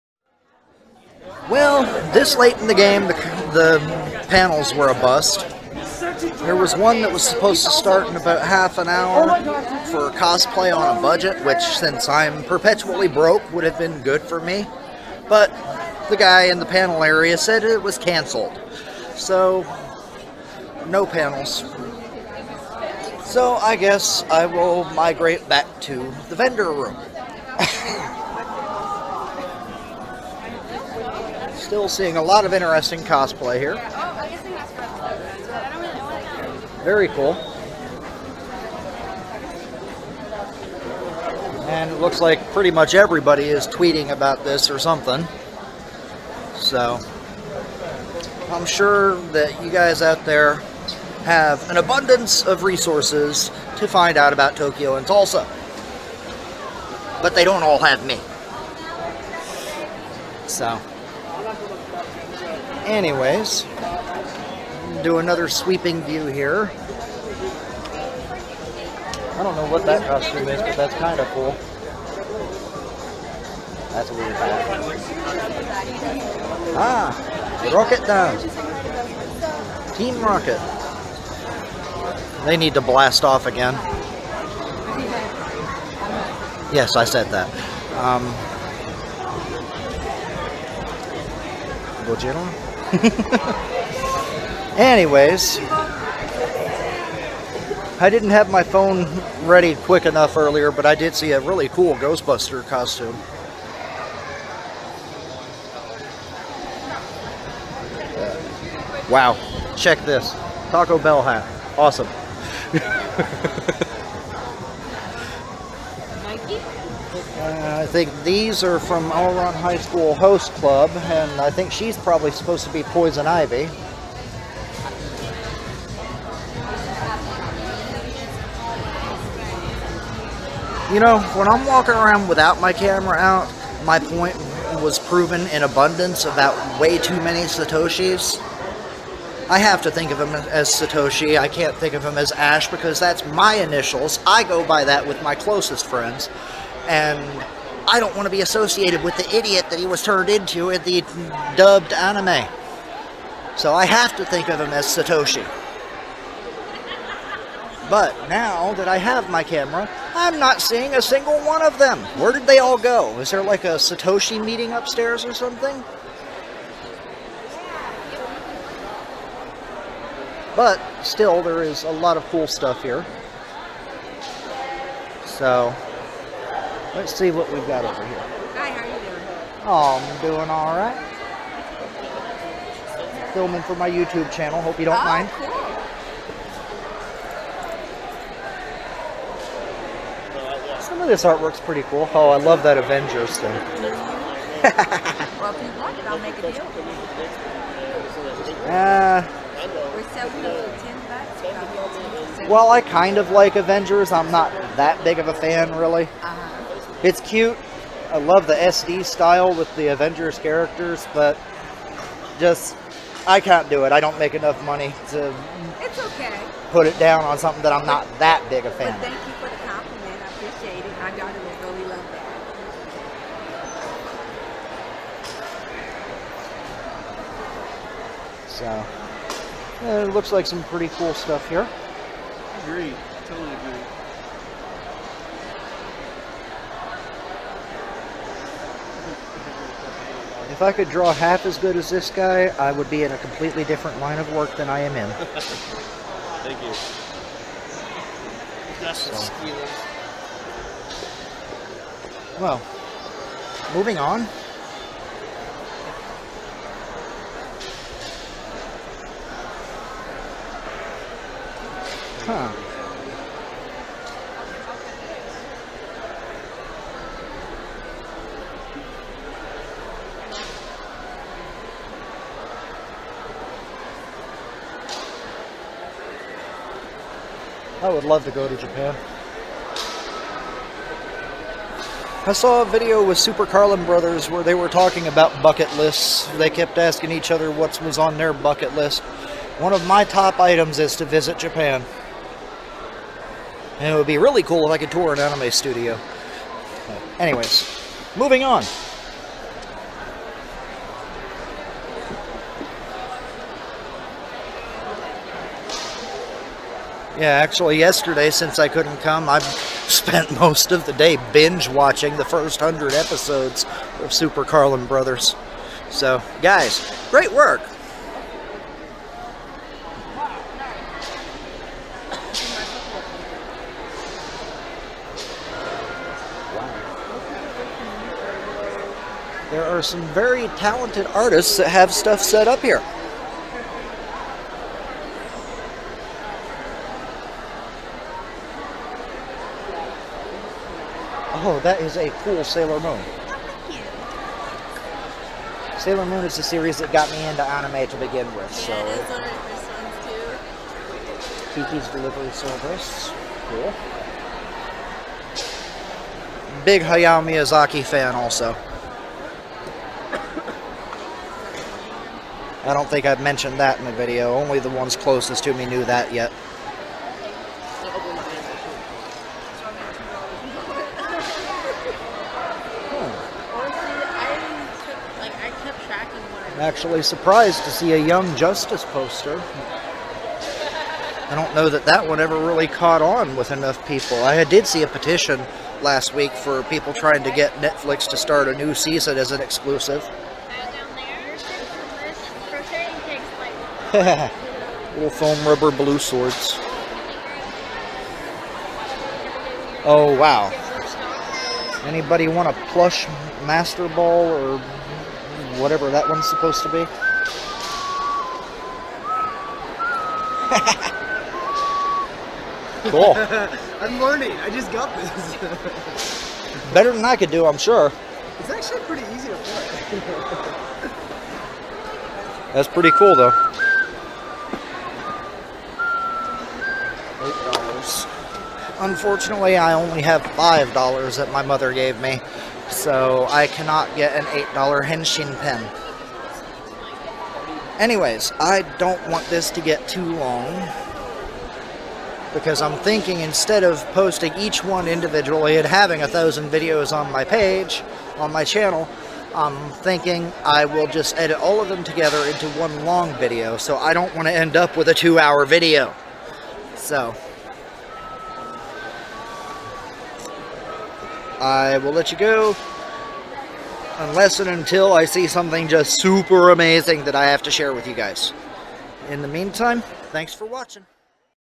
AOD Old School: Tokyo in Tulsa, Episode 7 (July 19, 2015)
Seventh part of my coverage from Tokyo in Tulsa 2015.

"More exploring, more cosplay, more cool stuff. I really enjoy Tokyo in Tulsa."